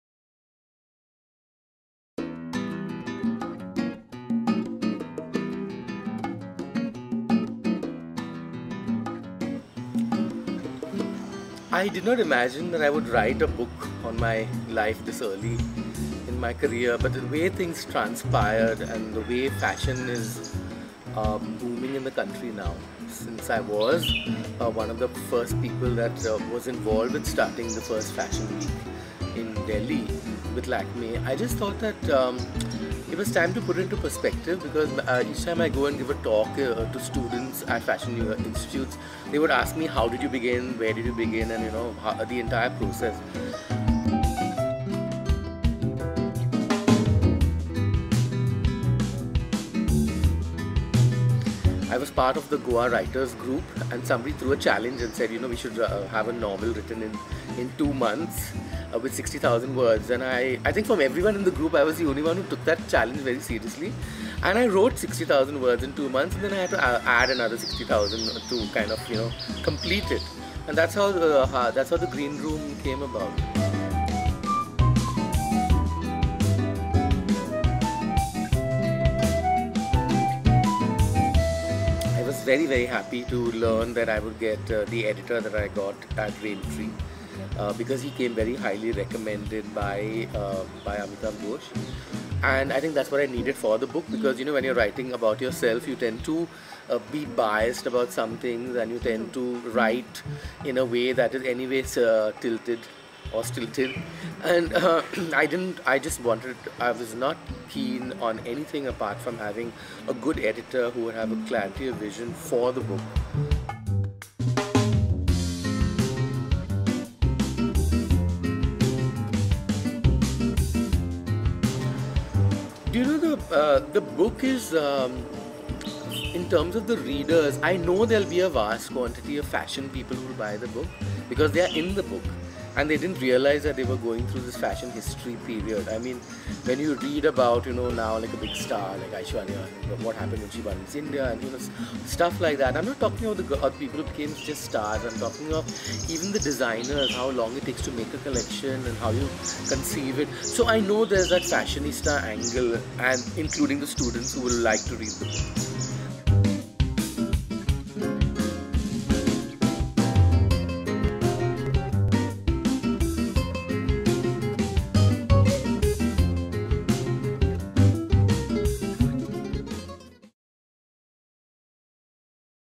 The Green Room by Wendell Rodricks
'The Green Room [is] a riveting read.' -- Amitav Ghosh

Peppered with wry observations and startling insights, The Green Room is not just the story of one man but also of the evolution of the Indian fashion industry over twenty- five years. It is both a deeply personal journey and a window into the enchanting yet fickle world of the rich and the famous, the beautiful and the neurotic, the gossips and the bores.

Candid, irreverent and unfailingly entertaining, this is a memoir of the man who gave India minimalism, resort wear and eco-friendly clothing long before these words became fashionable. It is a sparkling piece of writing that tells the story of a fascinating life lived with unusual courage and flair.